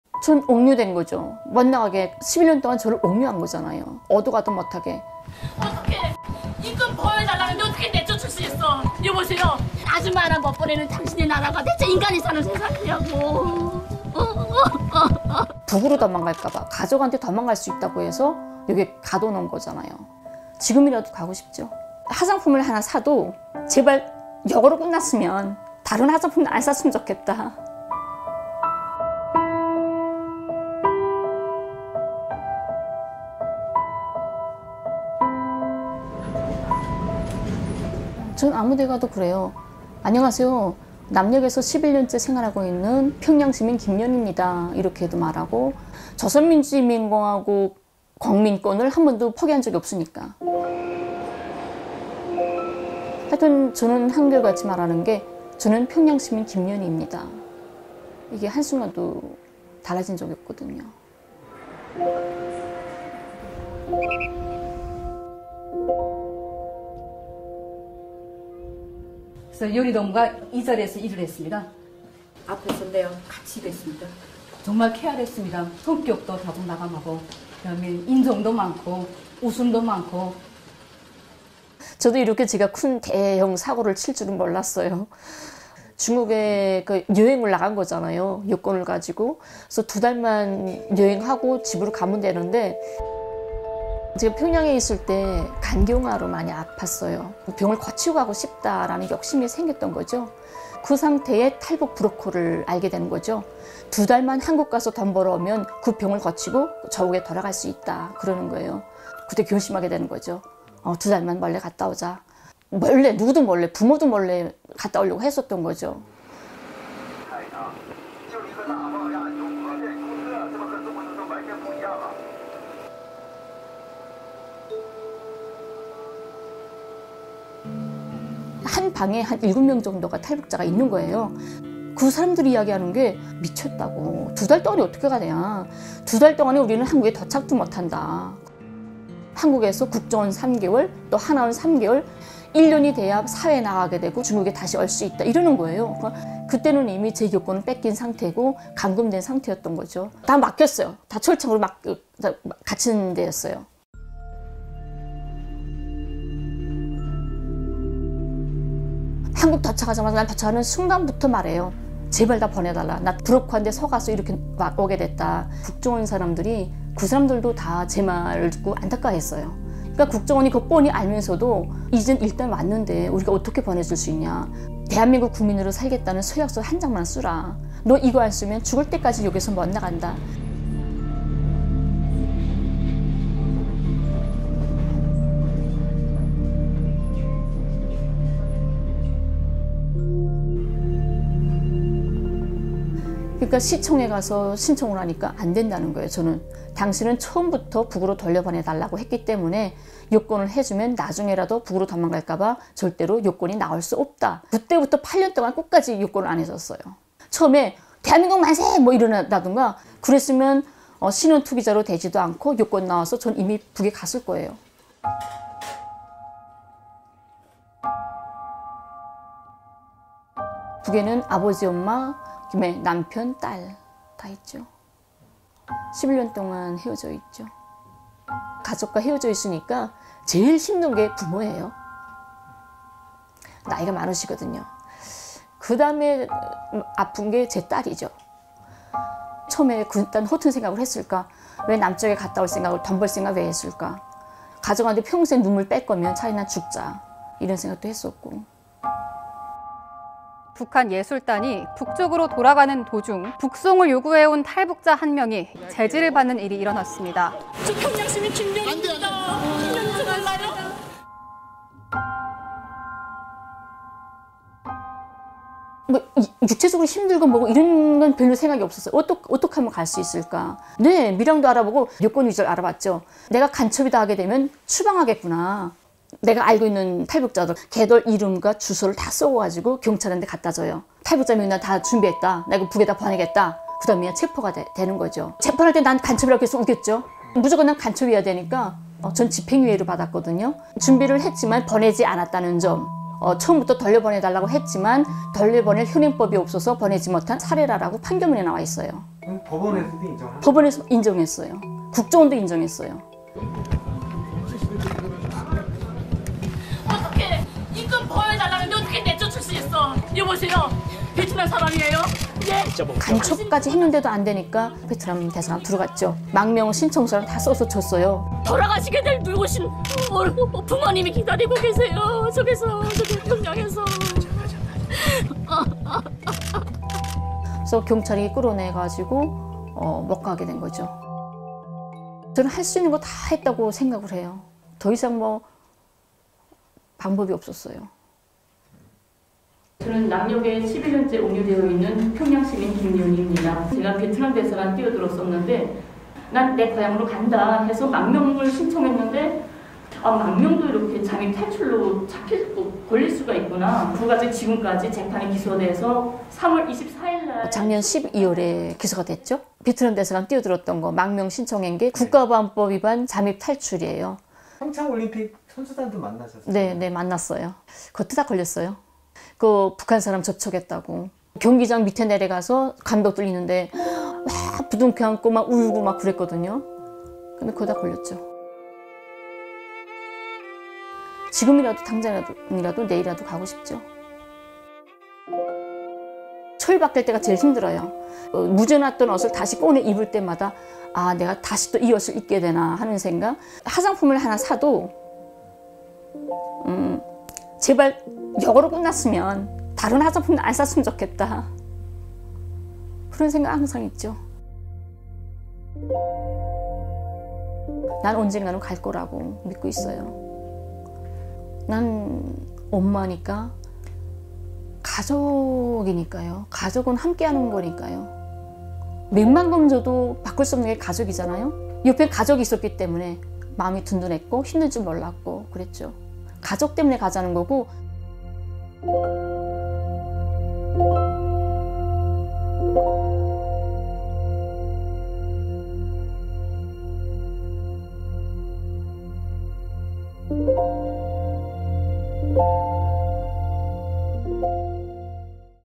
'나는 평양시민 김련희입니다' - BBC News 코리아
"출국금지가 풀리면 지금 당장이라도 (북한에) 가고 싶죠… 계절이 바뀌어 묻어둔 옷을 꺼낼 때마다 힘들어요. 화장품을 사도 이게 마지막이었으면 좋겠다..."

지난 2011년 9월 한국에 입국한 김련희씨. 그는 자신을 남한에 억류된 '평양시민'으로 소개한다.

고향을 등진 지 11년 째. 17살에 헤어진 딸은 이제 28살이 됐다. 몇 년 전 자신을 기다리던 나이든 어머니가 실명했다는 소식도 들려왔다.

2011년 5월 중국 여행 중 '한국에서 두 달 잠깐 일 하고 오면 큰 돈을 벌 수 있다'는 탈북 브로커의 말에 솔깃했다. 자신이 앓던 지병을 고치고 싶은 마음이 간절해 마음이 움직였다.

그는 그때까지만 해도 '탈북'이란 말도 몰랐고, 그저 한국으로 밀항해 두 달 일한 다음 금방 평양으로 돌아가리라로 순진하게 믿었다고 한다. 속은 걸 알았을 땐 이미 여권은 브로커에게 뺏기고 감금된 상태였다.

우여곡절 끝에 한국에 도착하자마자 그는 국정원 관계자에게 '브로커에 속아 왔으니 돌려보내 달라'고 호소했다. 하지만 한국에 온 이상 돌려보낼 방법이 없다는 말만 돌아왔다.

신원특이자로 분류돼 하나원 출소 후 여권도 발급돼지 않았다. 그때부터 여권 위조, 밀항을 알아보기도 했다. 북한으로 추방되리라는 기대로 스스로 간첩이라 주장하며 체포를 자처하기도 했다.

마지막 희망을 걸고 주한베트남대사관에 들어가 북한으로 망명을 신청했지만, 한국 경찰에 끌려 나와 그마저 수포로 돌아갔다.

검찰은 이를 국가보안법(잠입·탈출 등) 위반으로 보고 지난해 12월 그를 기소했다.

가족이 있는 북한으로 돌아가길 바라는 그녀의 11년 여정을 들어봤다.

기획·촬영·편집 최정민